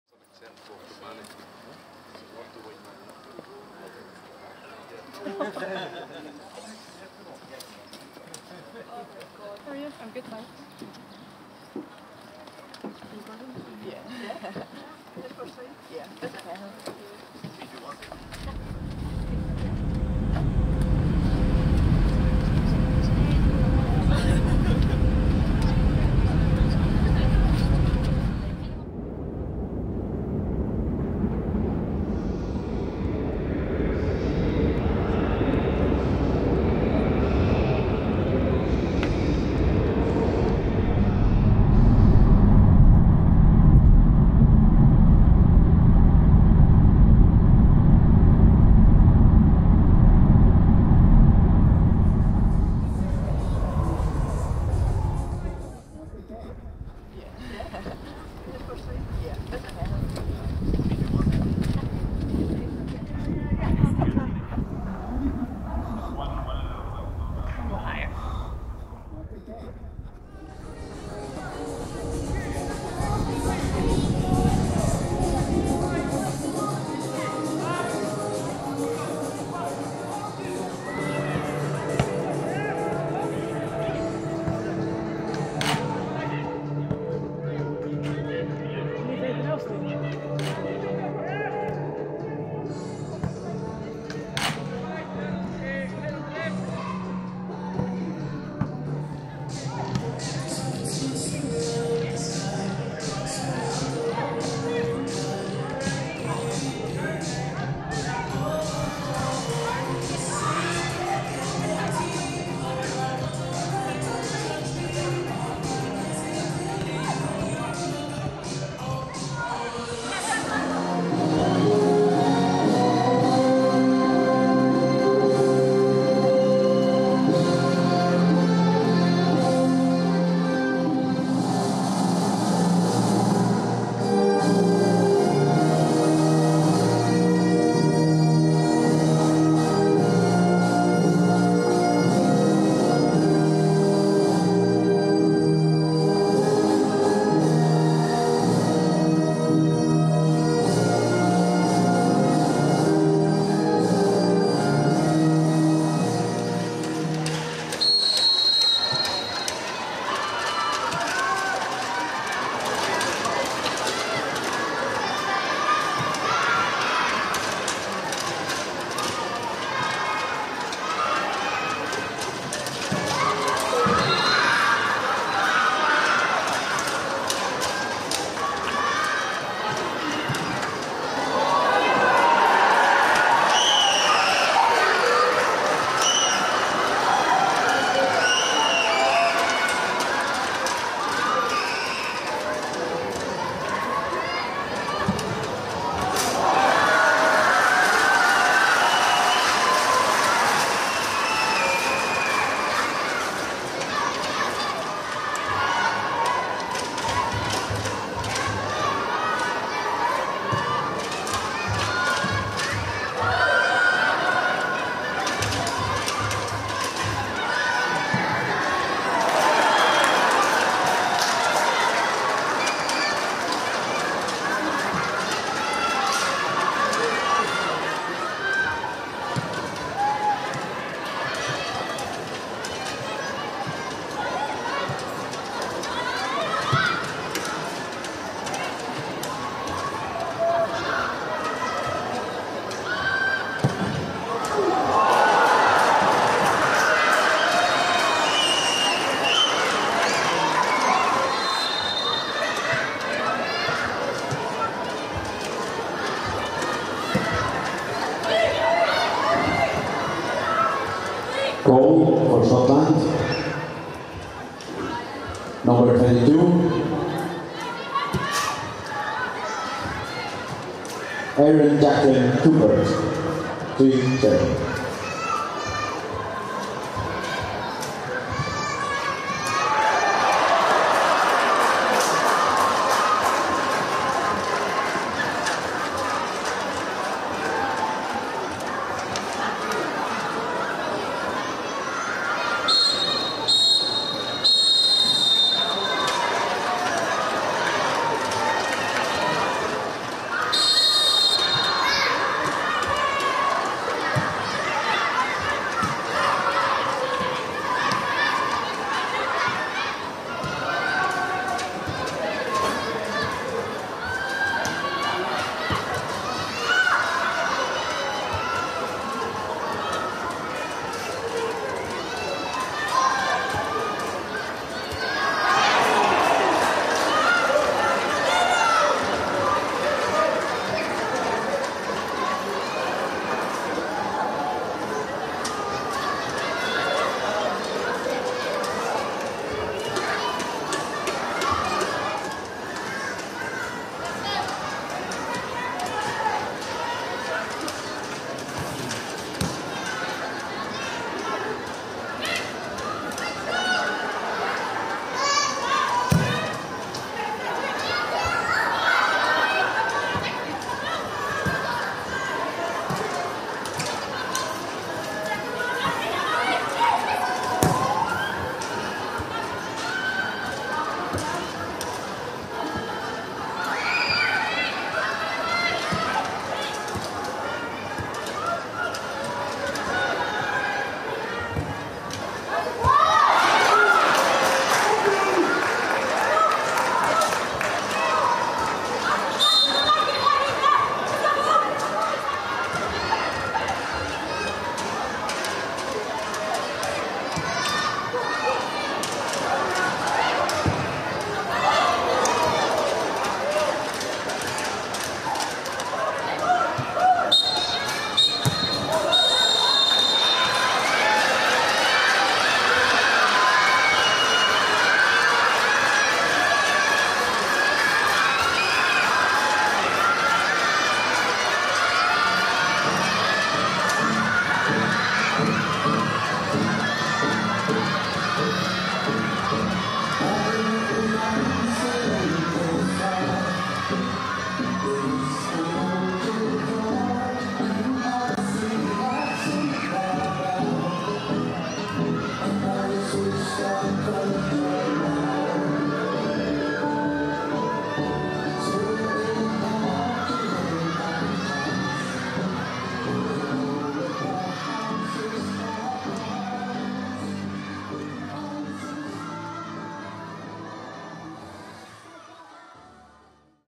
SWNT Sign off Qualification with 6 Goals! | Faroe Islands 0-6 Scotland | Scotland HQ View Highlights
➡️ Get your tickets for our Women's World Cup Qualifying Play-Offs

Scotland Women's National Team ended their camp on a high thanks to a 6-0 victory against the Faroe Islands, with goals from Nicola Docherty, Caroline Weir, Erin Cuthbert, Martha Thomas, captain Rachel Corsie and Jen Beattie.